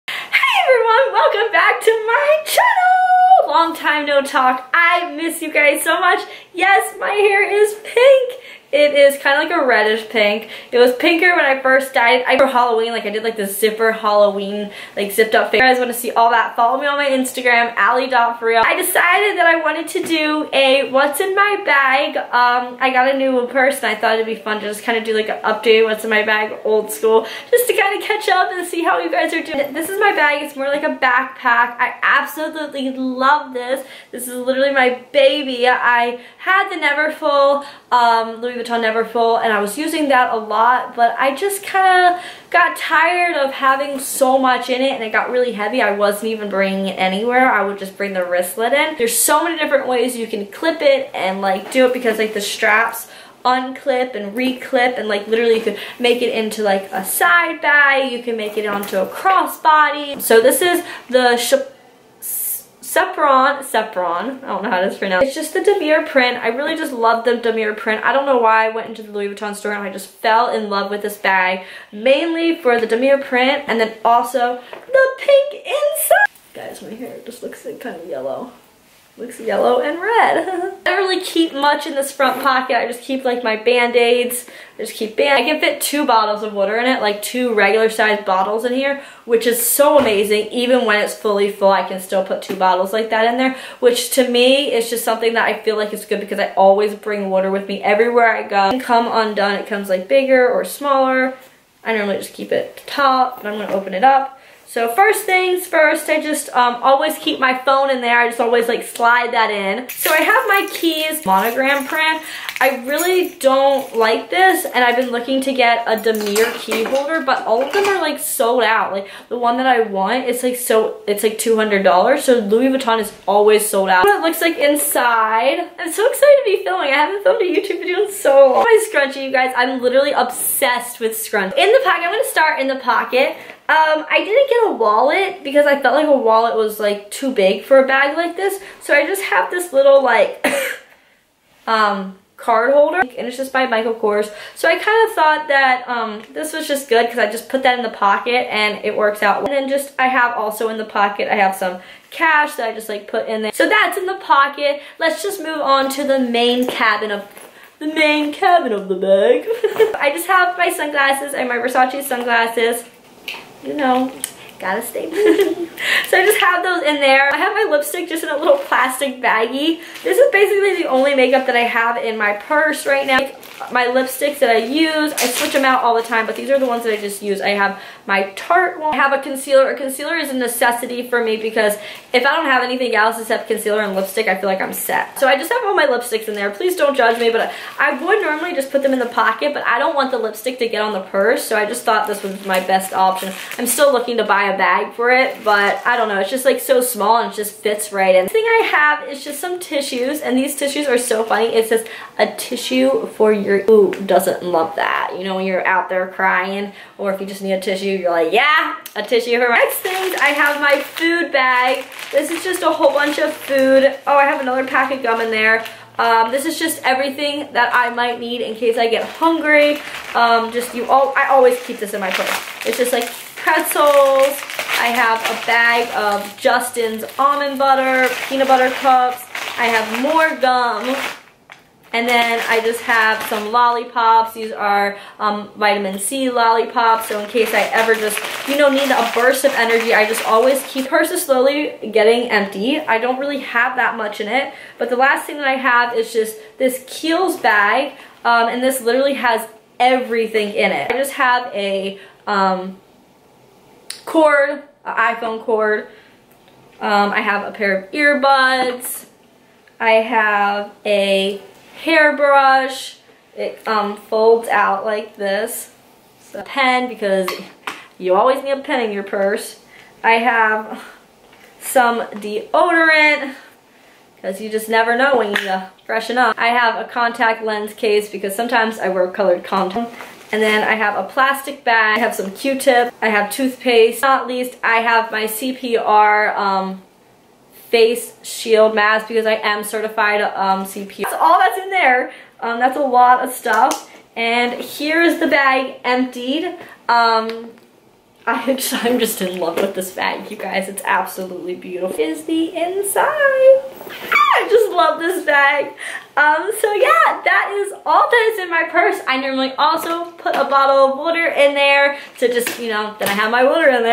I died my hair so many colors in the rainbow + WHATS IN MY Louis Vuitton Sperone
I miss you all so much!! I hope you all are doing amazing!!! IM SO EXCITED TO BE UPLOADING! I thought it would be fun to show you all what is in my Louis Vuitton Sperone. I hope you enjoy this video! I love you all so much!
xo
 Allie